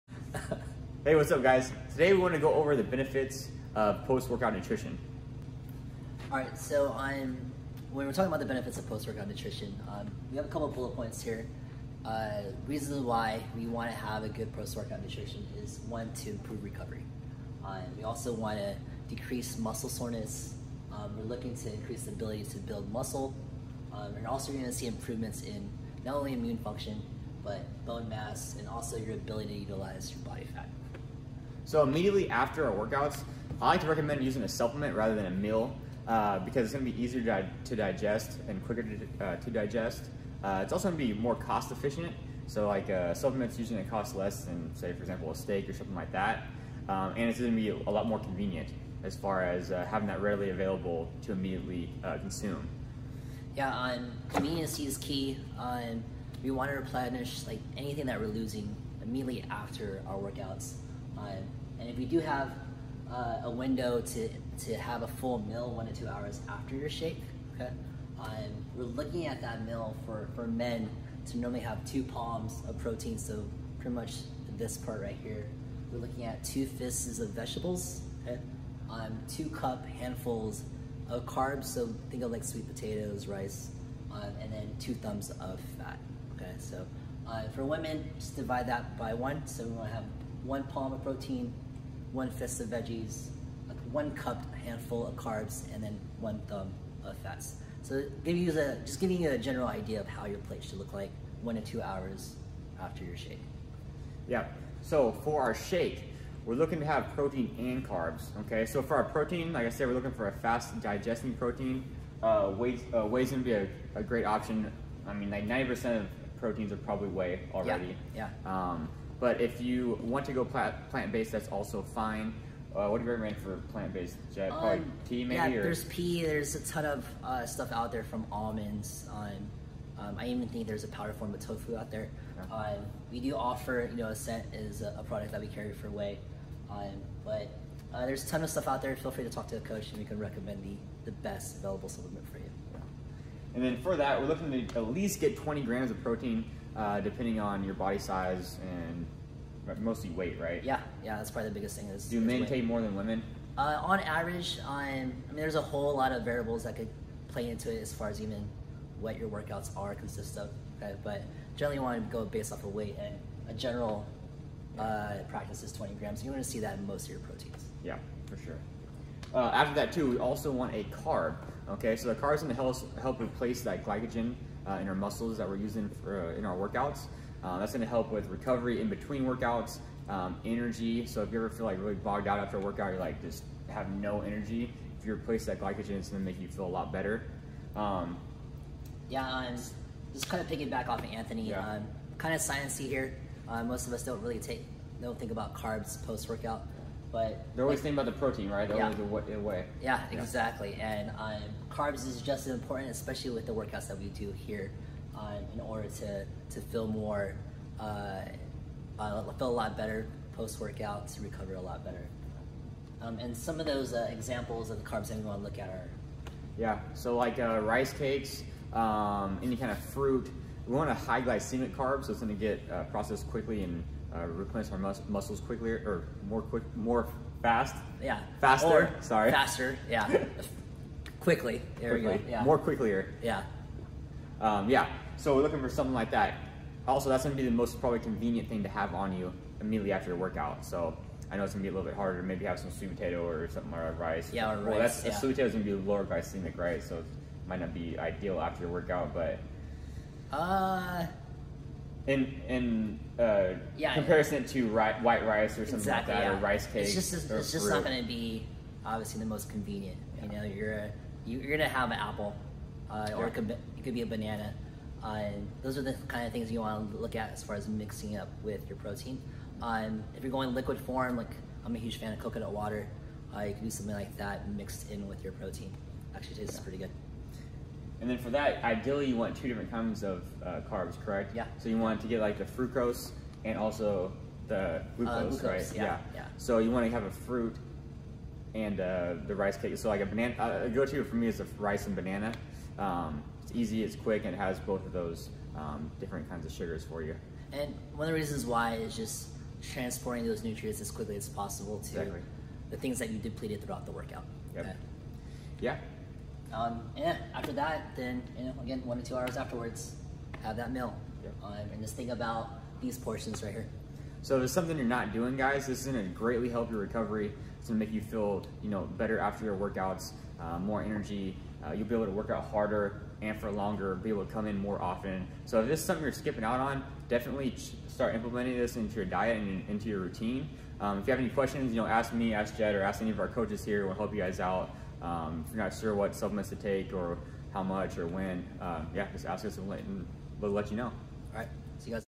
Post Workout Nutrition
🍎 𝐁𝐞𝐧𝐞𝐟𝐢𝐭𝐬 𝐎𝐟 𝐏𝐨𝐬𝐭 𝗪𝐨𝐫𝐤𝐨𝐮𝐭 𝐍𝐮𝐭𝐫𝐢𝐭𝐢𝐨𝐧 🍖

👆 Improved Recovery
👇 Less Muscle Soreness
💪 Improves Ability To Build Muscle
👩‍⚕️ Improve Immune Function
🍖 Improve Bone Mass

We prefer using a supplement rather than a meal immediately after your workout for your protein source.

Shakes are more convenient, cheaper, and digest quicker.

𝐏𝐫𝐨𝐭𝐞𝐢𝐧 20-40gs, whey or plant based
Aids in protein synthesis to help rebuild muscles after training. It will also reduce muscle soreness.

𝐂𝐚𝐫𝐛𝐬 40-60gs, higher glycemic is ideal, dextrose AND fructose is ideal.
Carbs will replenish glycogen in your muscles and liver that was used during your training. Carbs will increase your recover and energy between workouts.

We recommend eating a meal 1-2 hours after your workout after your initial shake. Here is a quick example of a meal with an easy to use measuring system:

𝐌𝐞𝐧:
2 Palms Protein
2 Fists Veggies
2 Cupped Handfuls Carbs
2 Thumbs Fats

𝗪𝐨𝐦𝐞𝐧:
1 Palm Protein
1 Fist Veggies
1 Cupped Handful Carbs
1 Thumbs Fats

🍌 A great example for your post workout supplement would be a protein shake along with a rice cake and banana.

While this isn't the perfect answer for everyone, it's a great start for anyone who isn't taking advantage of post workout nutrition at all.

There are a lot of small changes that could also be made to increase performance depending on your individual needs.